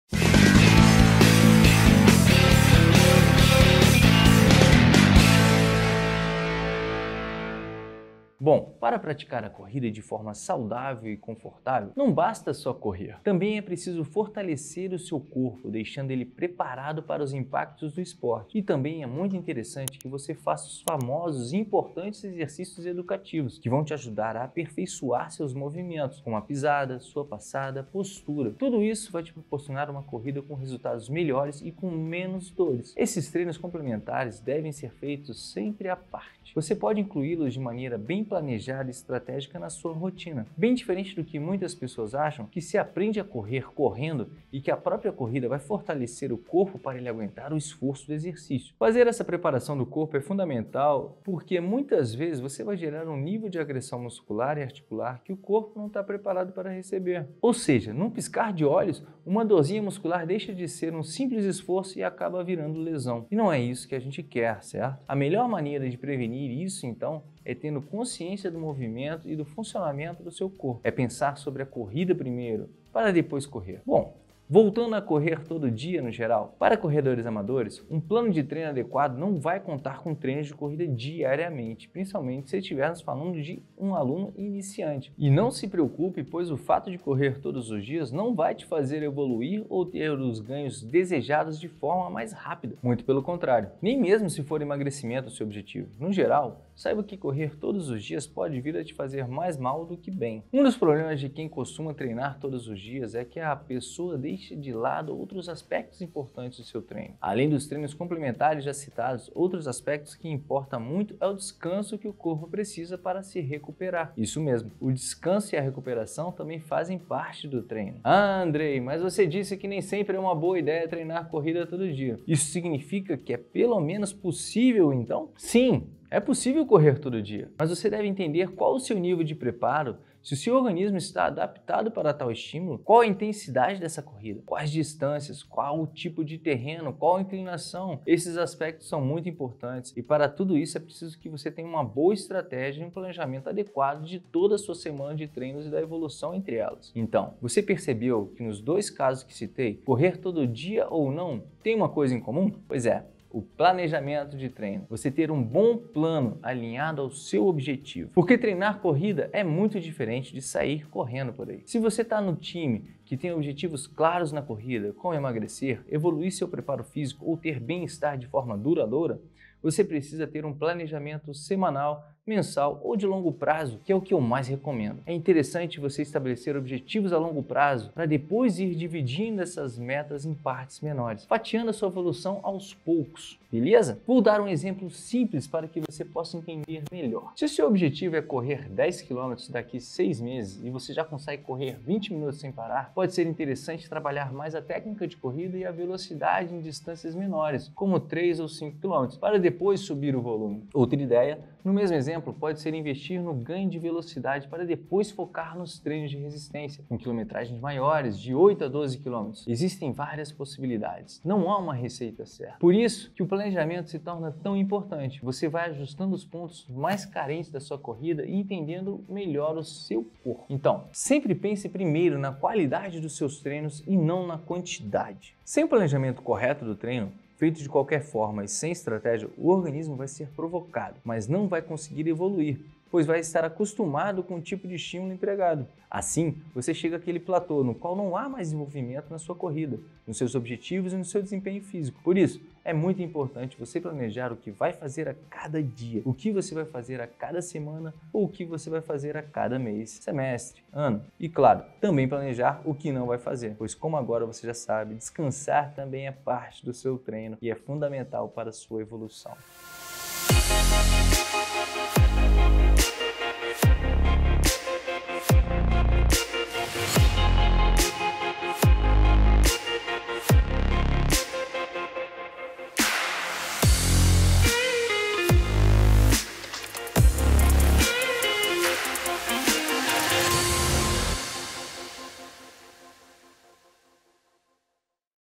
Correr TODO DIA: considere isso ANTES DE COMEÇAR para ter mais SEGURANÇA!
É possível correr todos os dias? Sim. Mas para fazer isso de forma segura e saudável, considere essas orientações do nosso head coach Andrei Achcar.

📲 Baixe GRÁTIS nosso aplicativo para acessar exercícios que vão te ajudar a correr melhor: